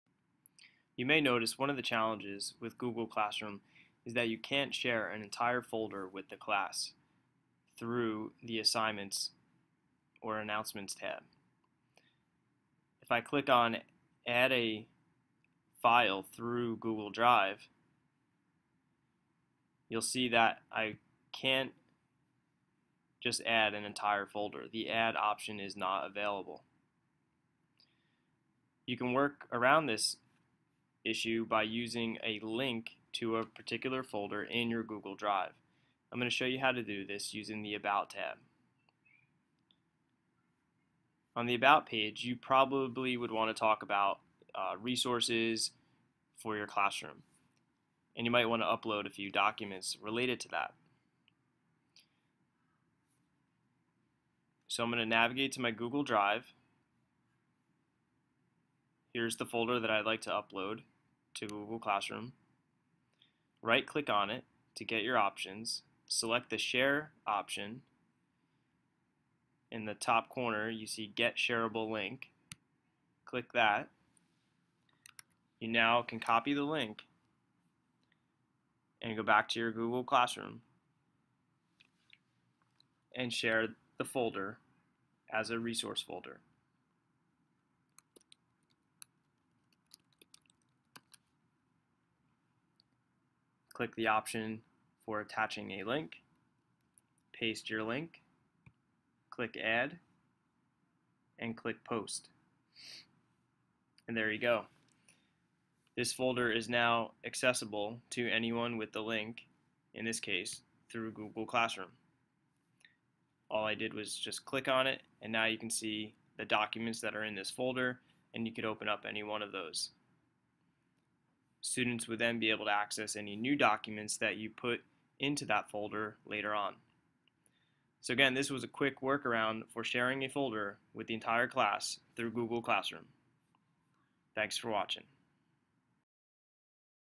Share a Folder through Google Classroom
A quick demo on how to share a folder through Google Classroom